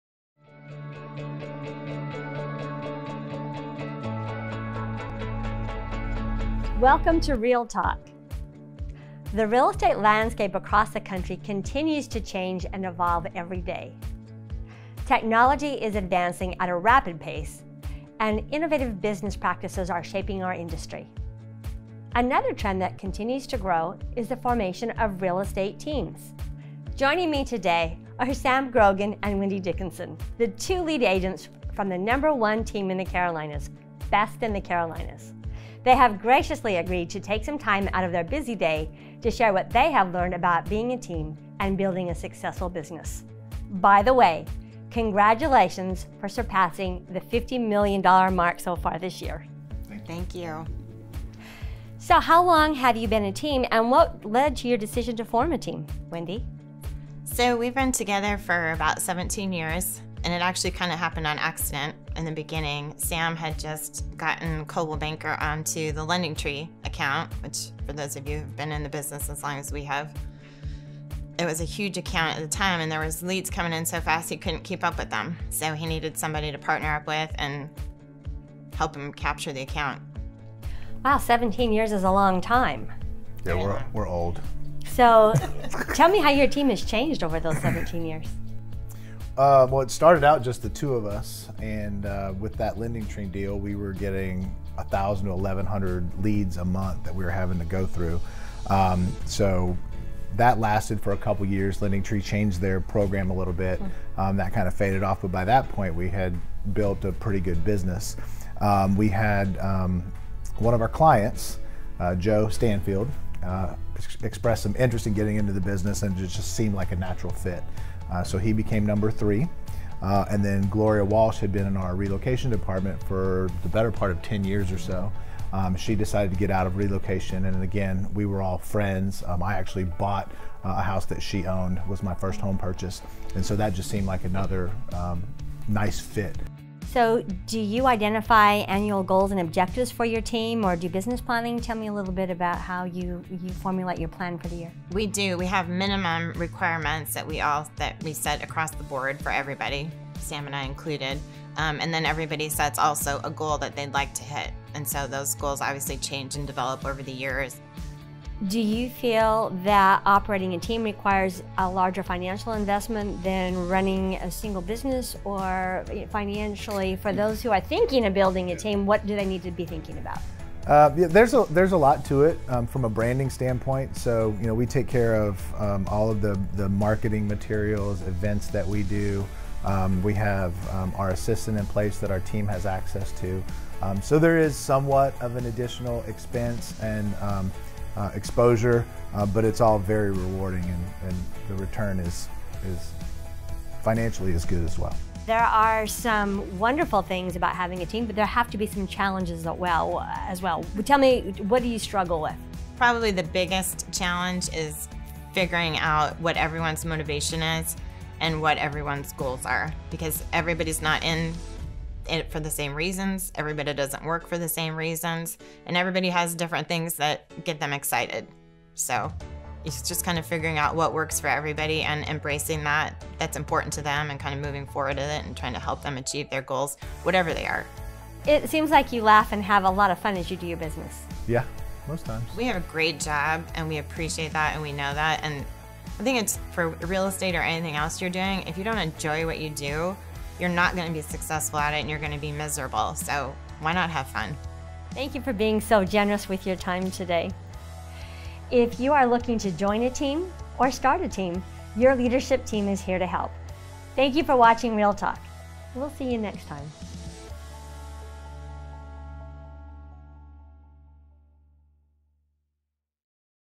#1 Large Team in the Carolinas, Best in the Carolinas
Learn how Sam Grogan and Wendy Dickinson, leaders of the #1 Large Team in the Carolinas, Best in the Carolinas, grew their successful business.